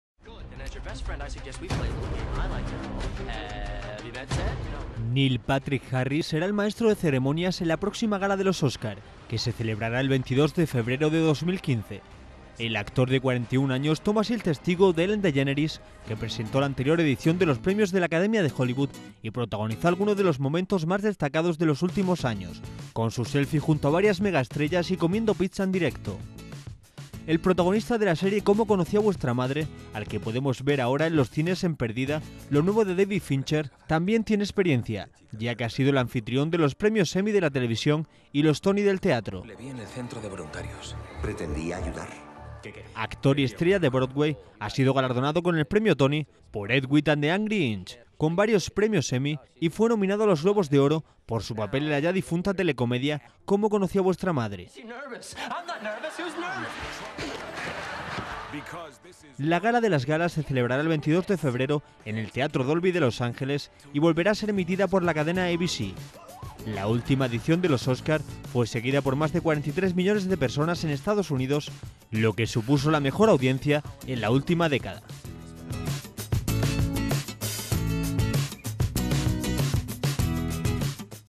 Neil Patrick Harris presentará los Oscar 2015
Neil Patrick Harris será el maestro de ceremonias en la próxima gala de los Oscar, que se celebrará el 22 de febrero de 2015. El actor, de 41 años, toma así el testigo de Ellen DeGeneres, que presentó la anterior edición de los premios de la Academia de Hollywood y protagonizó algunos de los momentos más destacados de los últimos años con su selfie junto a varias megaestrellas y comiendo Pizza en directo